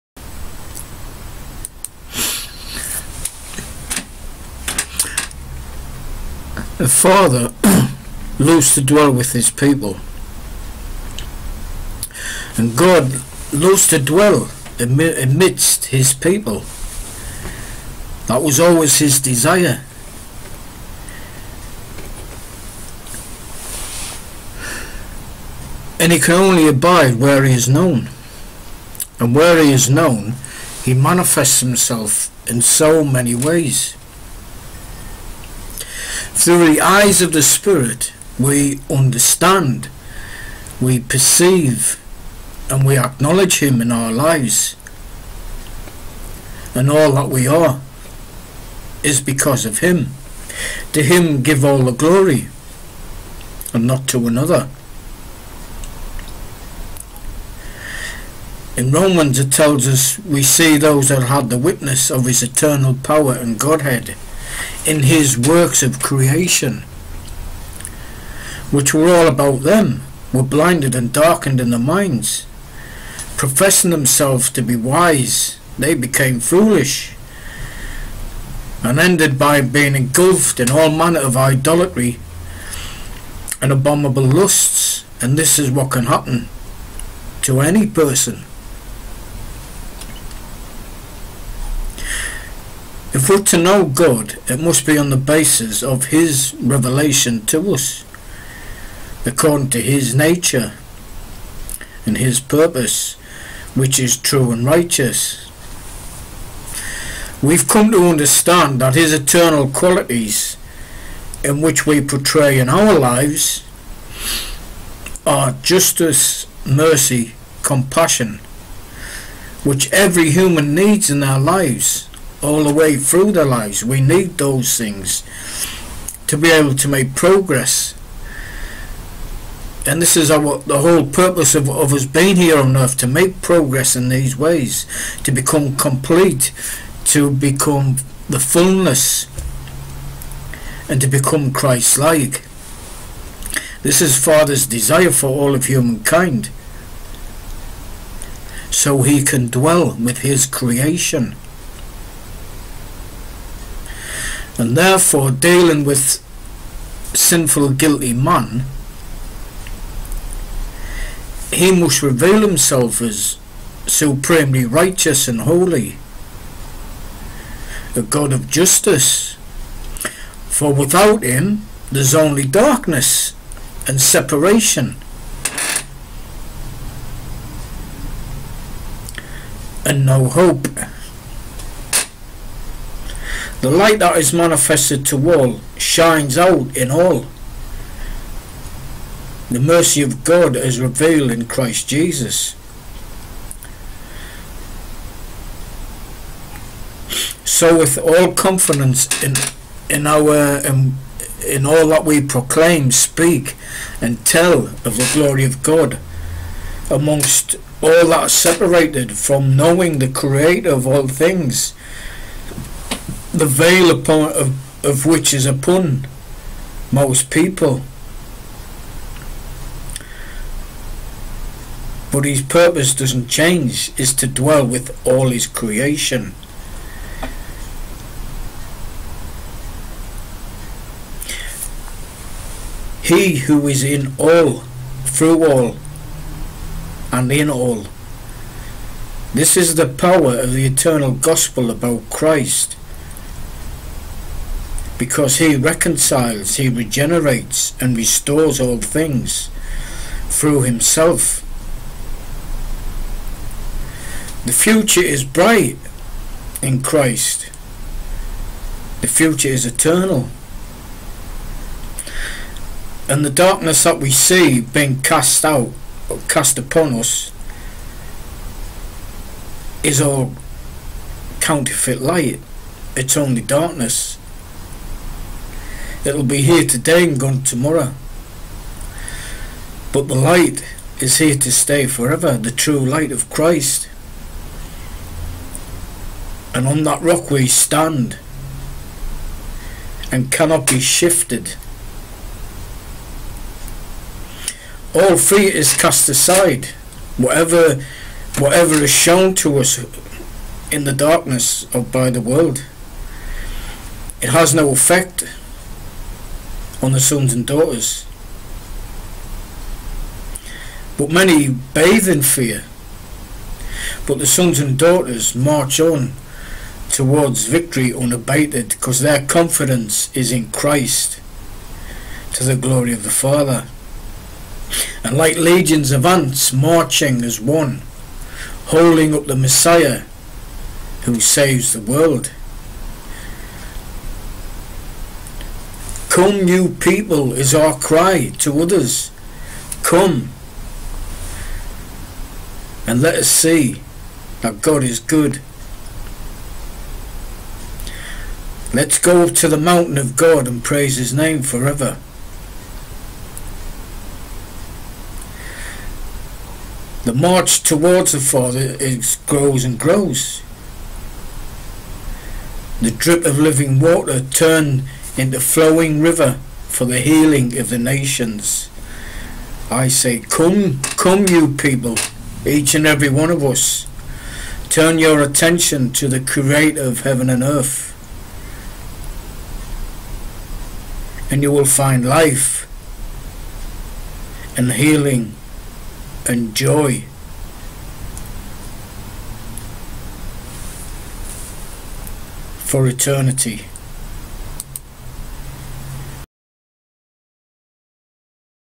Almighty God loves to dwell with his Creation
It's always been fathers desire to dwell with his creation and in his wisdom he made the way for him to dwell within us